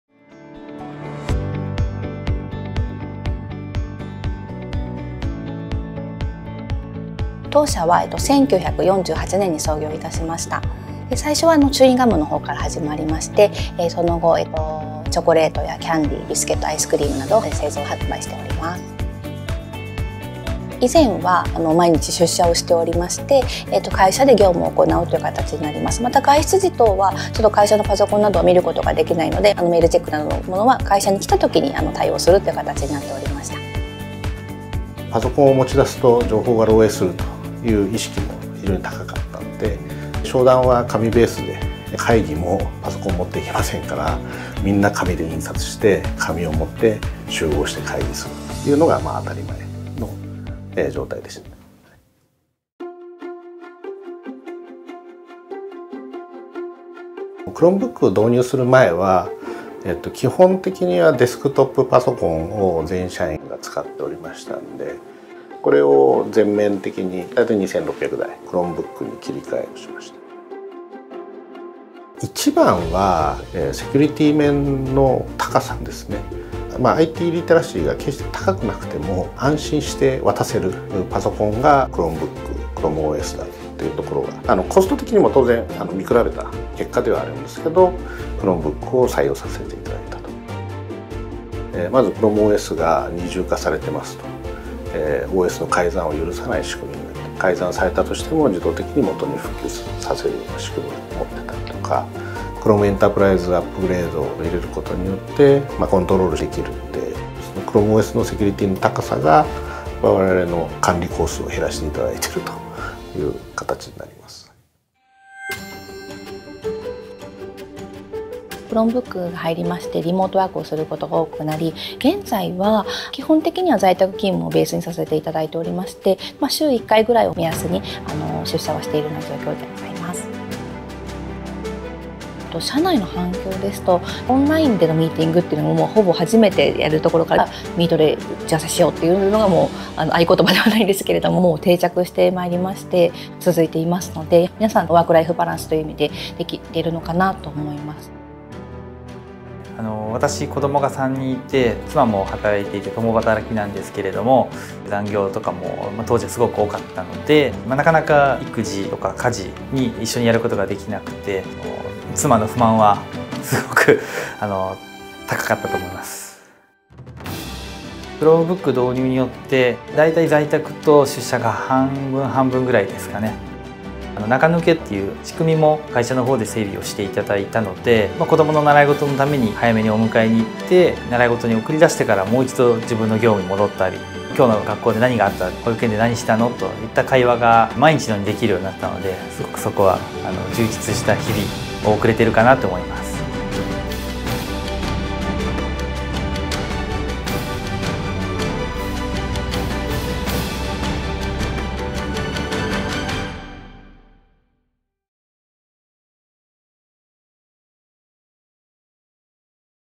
株式会社ロッテが ChromeOS で新しいワークスタイルを確立
菓子・アイス製造販売の株式会社ロッテは、2020年に DX の一貫として Chromebook 2,600台を導入。ChromeOS のセキュリティの高さが組織の変革に寄与し、Chromebook を使ったテレワークの導入が実現。これまで全社員がデスクトップパソコンだった状況から、ワークライフ バランスのとれた職場環境へと変貌を遂げました。
※動画内の情報は 2023年9月時点のものです。